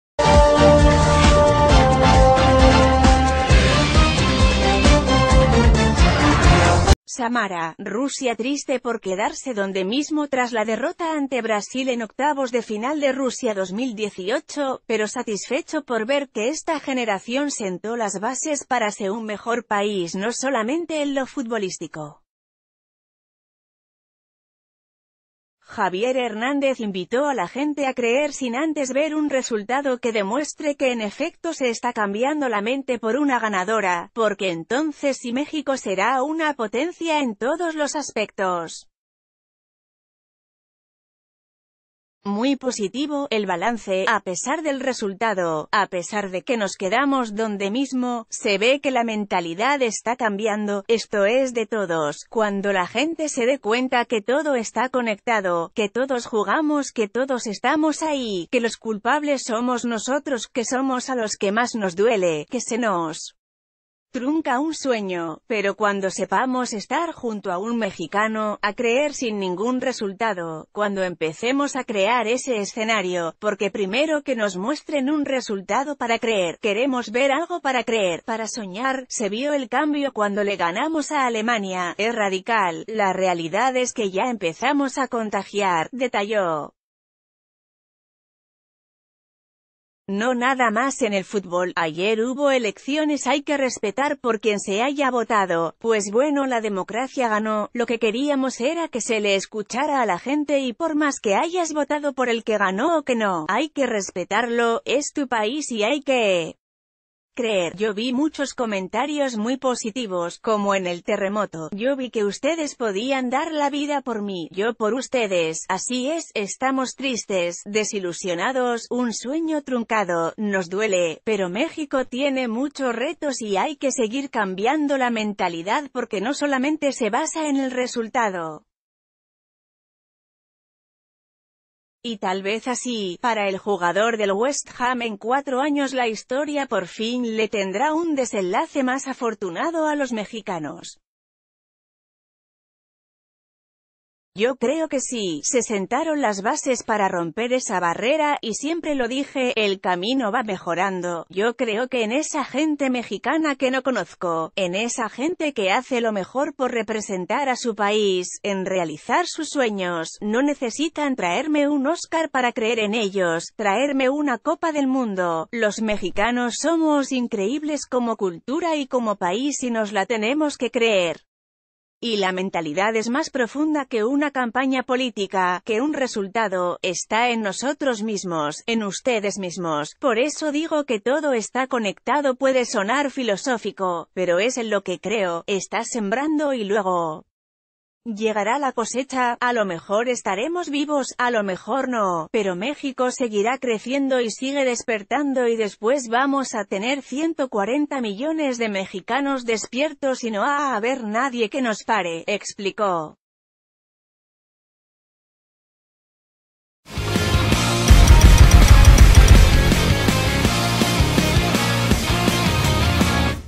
Chicharito pide creer en el Tri a ciegas, sin esperar resultados
SAMARA, Rusia -Triste por quedarse donde mismo tras la derrota ante Brasil en octavos de final de Rusia 2018, pero satisfecho por ver que esta generación sentó las bases para se un mejor país no solamente en lo futbolístico. Javier Hernández invitó a la gente a creer sin antes ver un resultado que d...